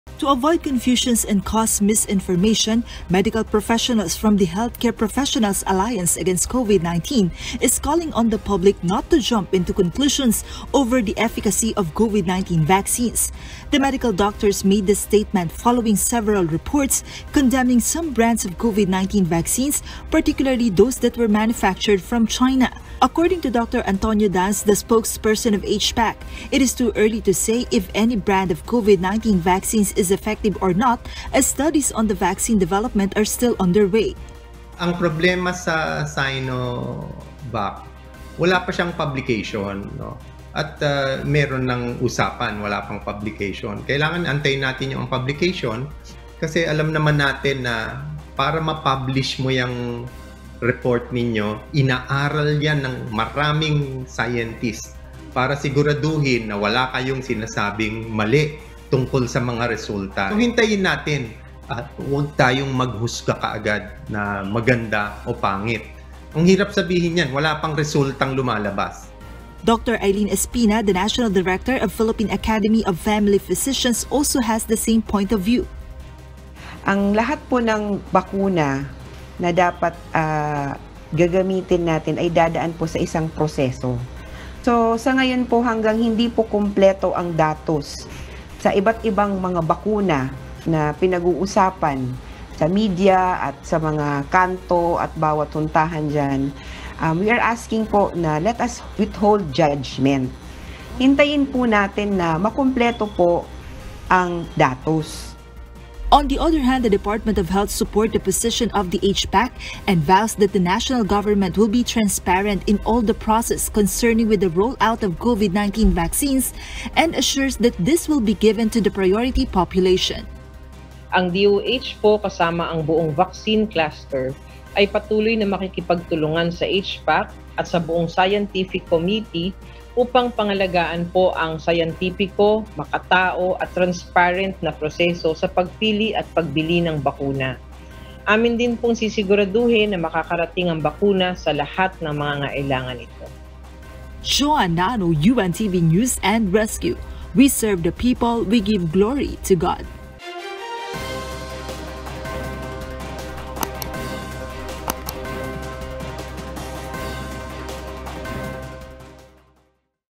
Experts appeal to the public refrain from judging COVID-19 vax, instead wait for scientific results
A group of medical experts appeal to the public to refrain from judging any brand of COVID-19 vaccines available in the market but instead wait for the scientific results.